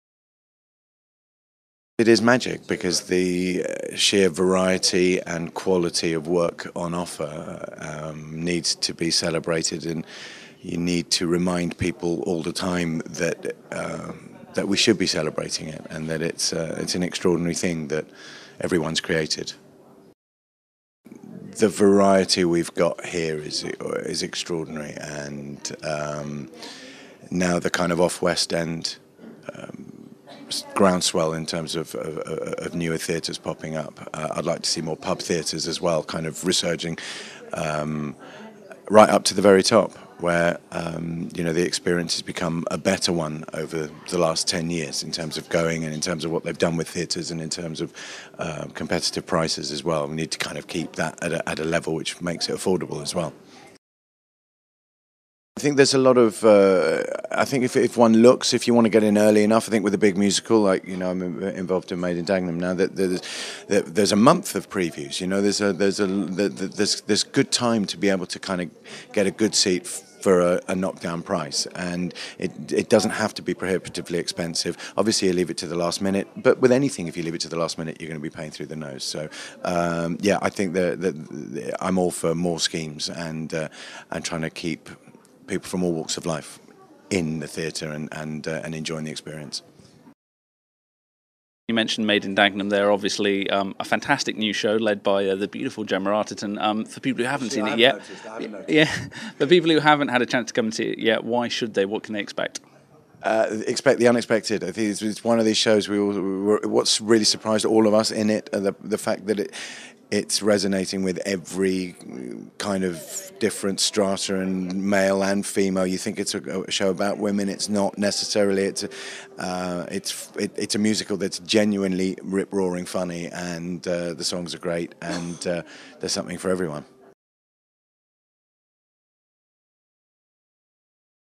Steve Furst Interview - Made In Dagenham - What's On Stage Awards
Red Carpet News talks to the stars of the West End at the nominations announcement for the What's On Stage Awards, celebrating the best in dramatic and musical theatre.